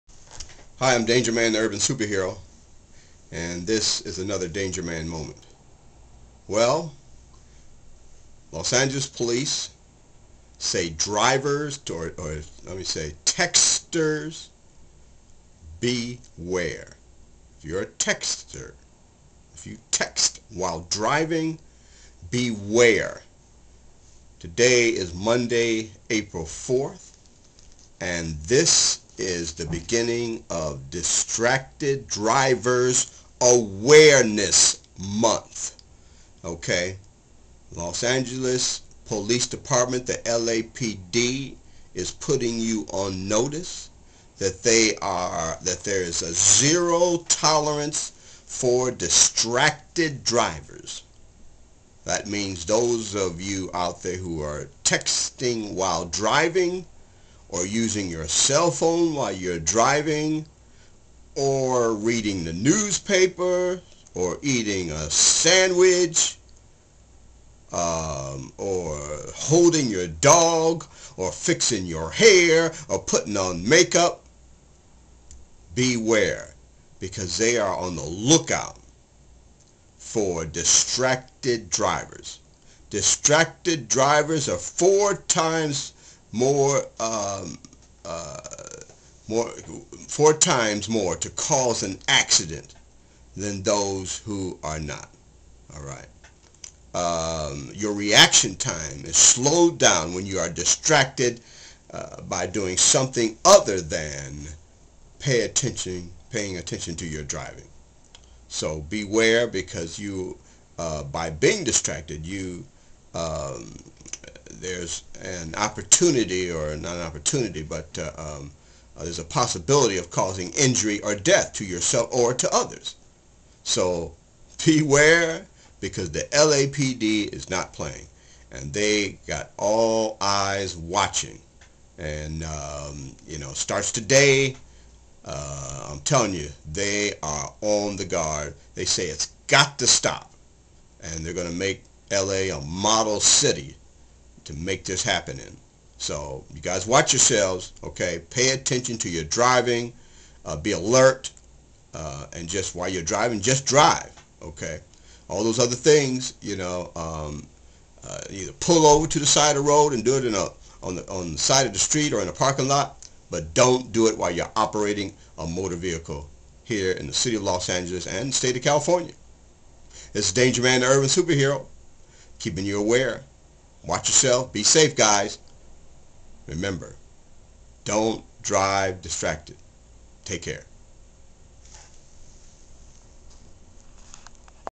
Texter's Beware
The LAPD today kicked off Distracted Driver Awareness Month, they are taking a ZERO tolerance approach, on texting,cell phone use,eating your favorite burger, sleeping, having sex,holding your dog, putting on make up. Distracting driver cause injury or death.